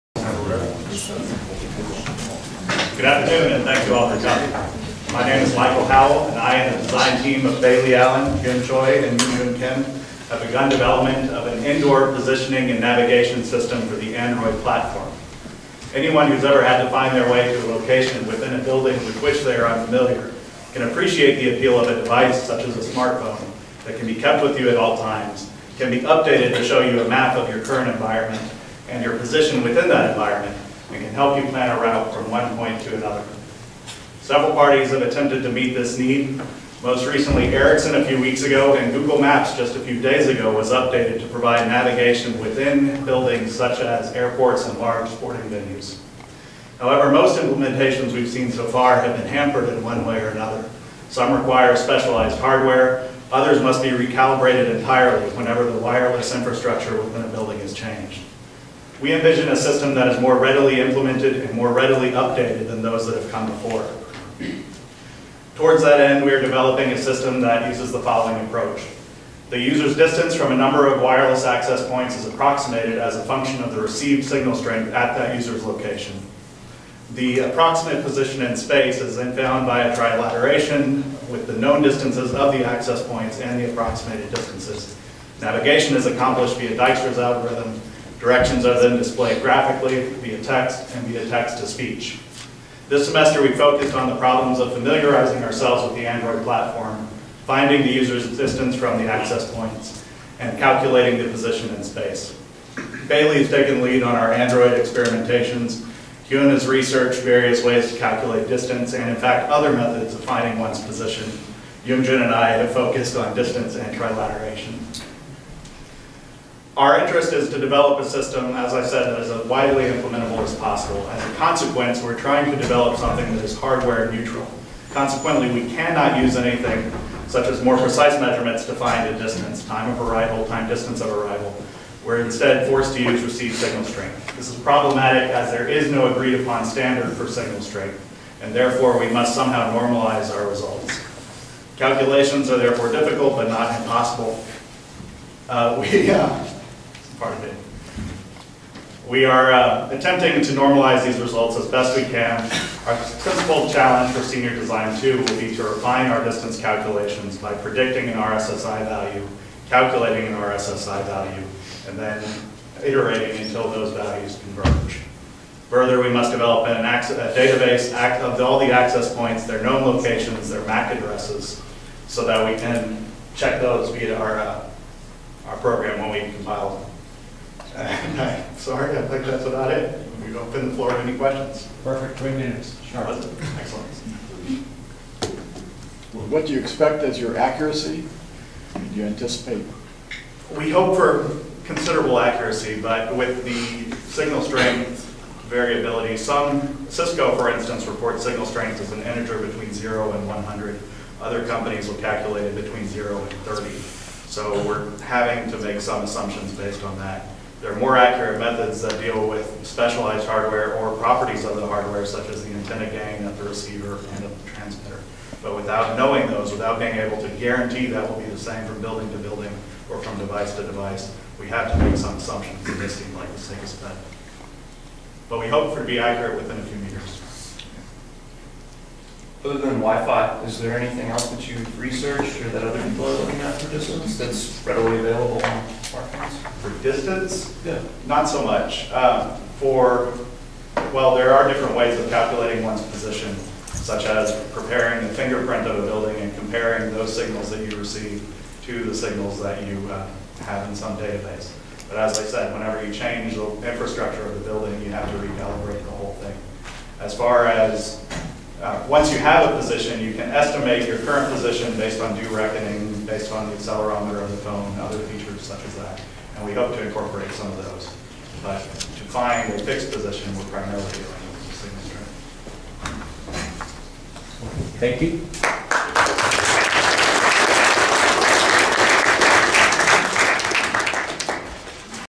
WiFi Based Positioning System on the AndroidTM Platform
SDDF2011 Team1
Title: WiFi Based Positioning System on the AndroidTM Platform

Abstract:  This project is intended to create a hardware-neutral indoor positioning and navigation system. Hardware neutrality is necessary for the development of a solution that may be implemented in a broad range of environments and may be used by a wide variety of Android devices. We have developed an approach that uses detected Wi-Fi signals to identify fixed reference positions and to estimate the distance between the user and those positions.  An approximate position in space may be calculated through the use of a trilateration algorithm and navigation between any two points is made possible with the aid of Dijkstra's algorithm.  We will describe our approach to positioning and navigation and discuss the limitations of our solution.

-December 2, 2011, TI Auditorium, UT Dallas